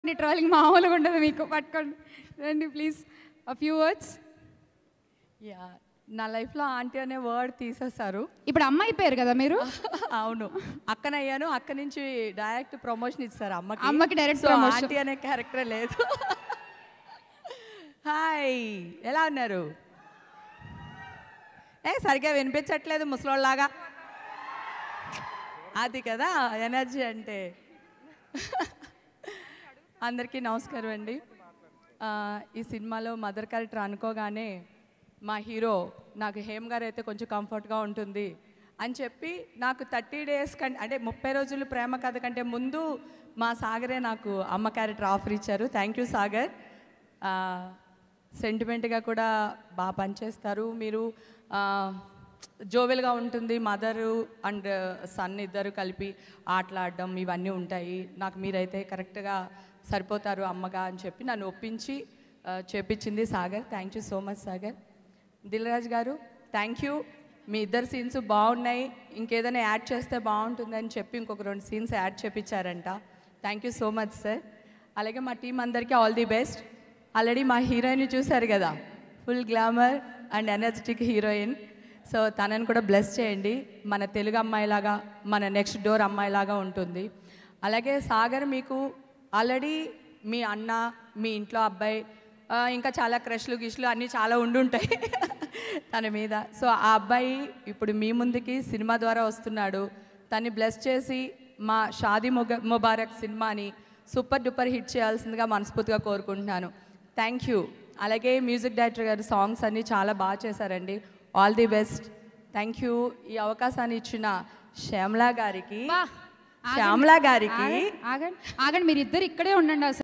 Hema Funny Speech | Shaadi Mubarak Pre Release Event | Dil Raju | Sagar RK Naidu | Drishya Raghunath
Hema Funny Speech, Shaadi Mubarak Telugu Movie Pre Release Event LIVE on Telugu FilmNagar. ShaadiMubarak Latest 2021 Telugu Romantic Comedy Movie ft. Sagar RK Naidu & Drishya Raghunath. Music by Sunil Kashyap. Directed by Padmasri. Produced by Dil Raju and Shirish under Sri Venkateswara Creations banner. SagarRKNaidu​​ DrishyaRaghunath SunilKashyap DilRaju ShaadiMubarak​​

Movie Details:
Movie- Shaadi Mubarak
Cast - Sagar RK Naidu, Drishya Raghunath
Director - Padmasri
Producers - Dil Raju, Shirish
DOP - Srikanth Naroj